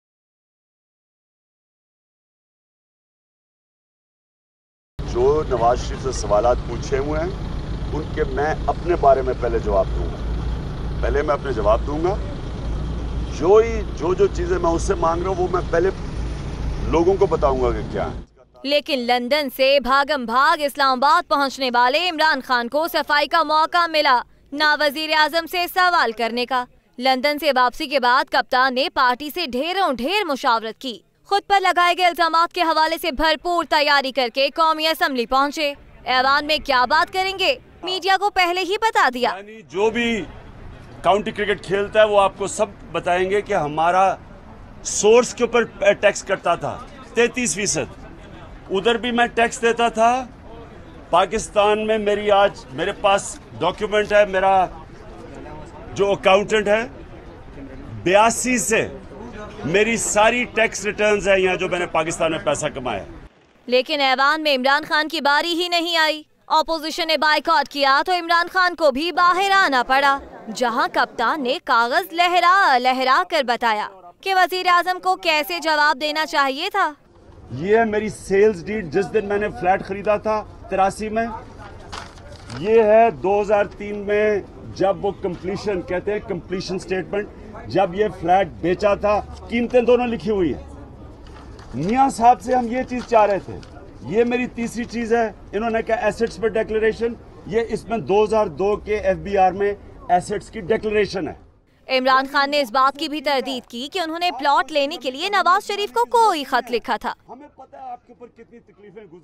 24 Report : Imran Khan proves his innocence in offshore companies matter
24 Report : Imran Khan proves his innocence in offshore companies matter